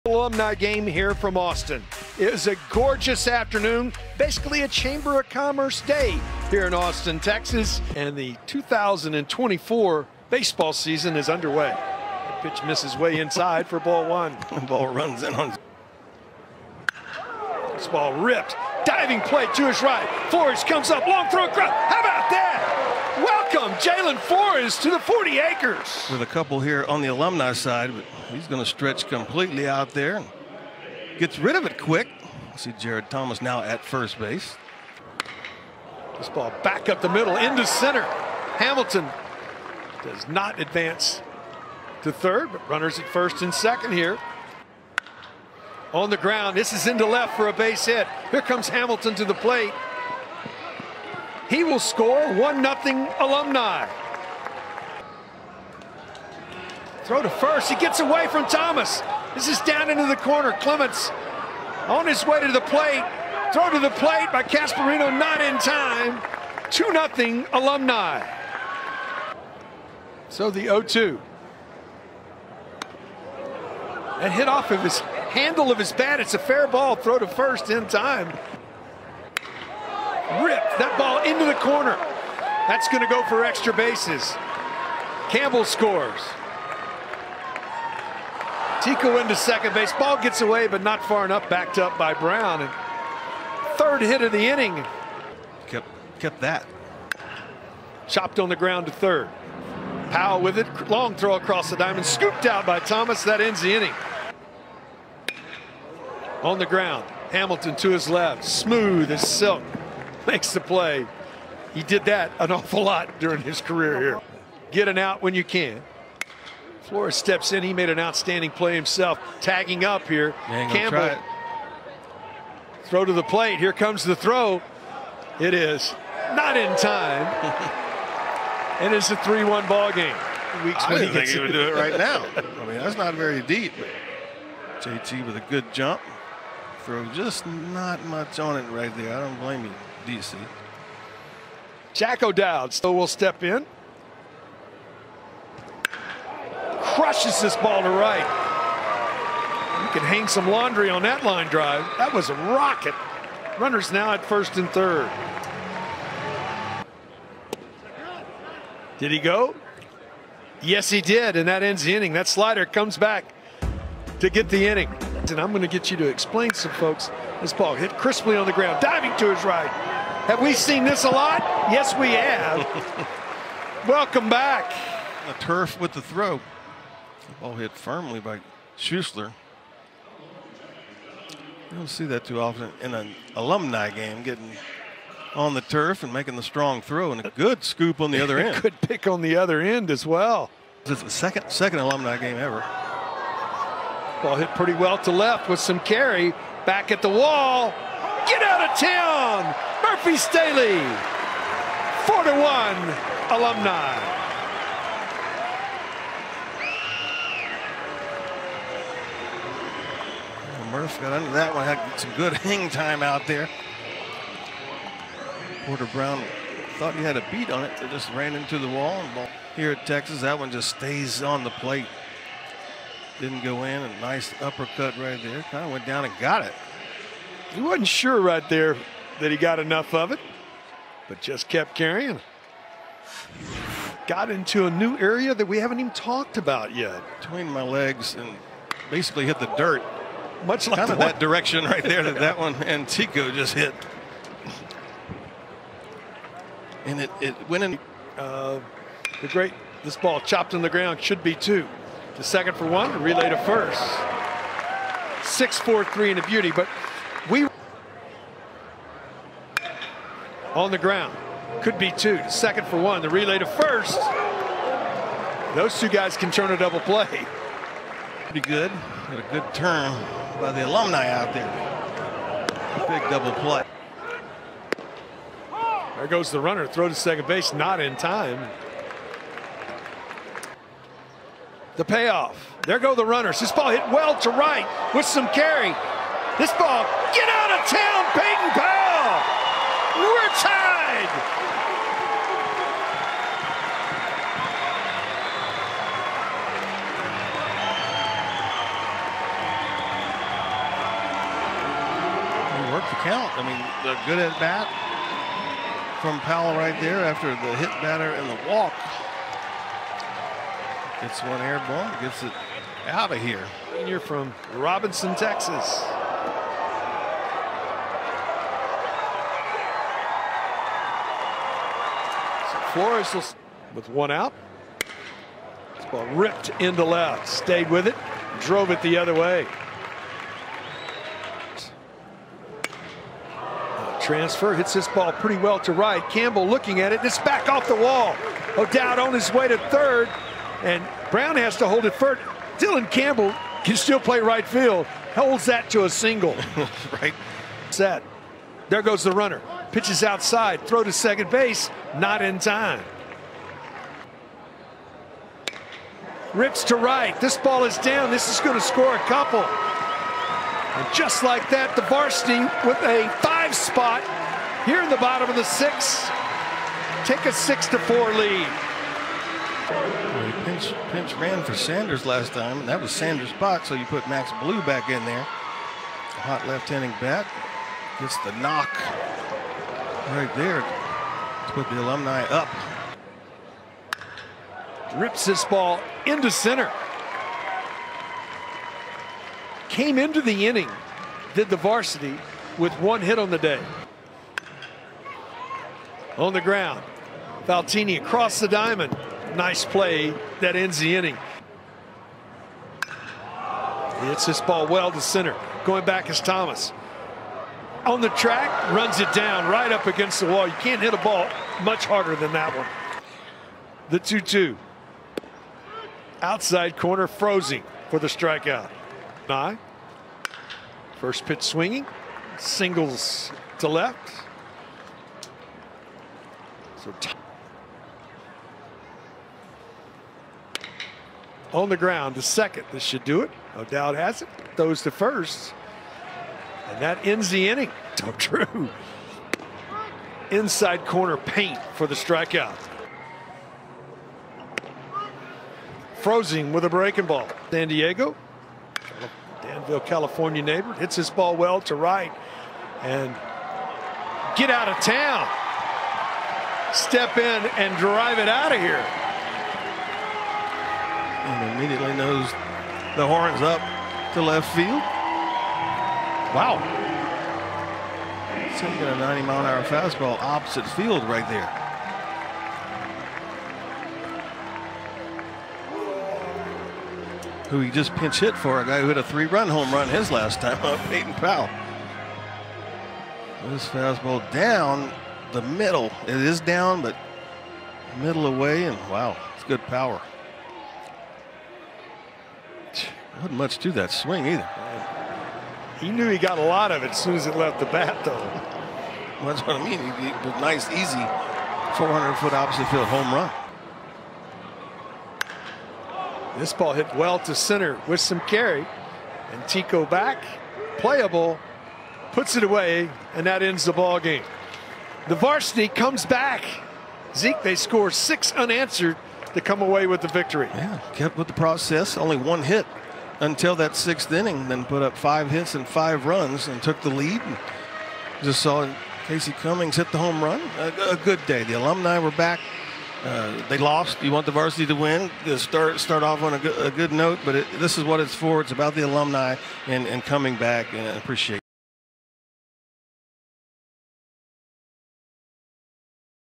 2024 Texas Longhorns Alumni Game | NCAA Baseball Highlights | Preseason Game | Austin
The Texas Longhorns baseball team hosted their annual alumni preseason game in Austin Texas on Saturday. The preseason game featured some of the Longhorn's most accomplished players that have moved on to professional baseball. The 7 inning game showcased some amazing offensive plays and the hard-nosed defensive skill that we have come to expect from the Texas Longhorns. Casey Cummings followed up his electric talent from the Fall World Series tournament with another late homerun to help seal the win for his squad. This is sure to be an exciting spring season with the Longhorns. Go Horns!